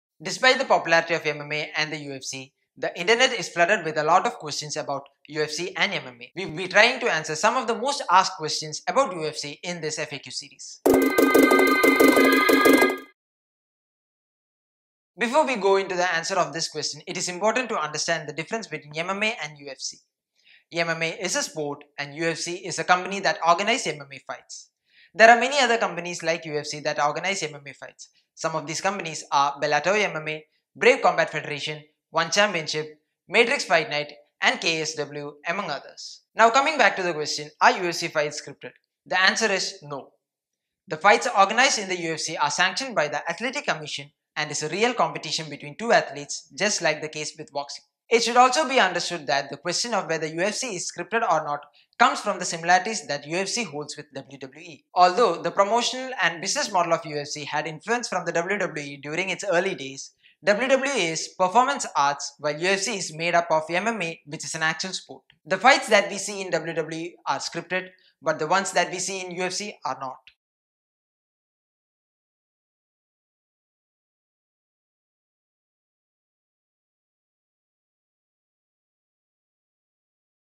FAQ EP: 03- Are UFC Fights Scripted? Are UFC fights fixed? Is UFC Real? | LockerRoom India | MMA
Are UFC Fights Scripted? Are UFC fights fixed?

Before we go into the answer to this question, it is important to understand the difference between MMA and UFC. MMA is a sport. UFC is a company that organizes MMA fights. There are many other companies like UFC that organize MMA fights. Some of these companies are Bellator MMA, BRAVE Combat Federation, ONE Championship, Matrix Fight Night, and KSW among others.

Now, coming back to the question: Are UFC fights scripted? The answer is no. The fights organized in the UFC are sanctioned by athletic commissions and is a real competition between two athletes just like the case with Boxing.

It should also be understood that the question of whether UFC is scripted or not comes from the similarities that UFC holds with WWE. Although the promotional and business model of the UFC had influence from the WWE during its early days, WWE is performance arts while UFC is made up of MMA which is an actual sport.

The fights that we see in WWE are scripted but the ones that we see in UFC are not. The answer is as simple as that.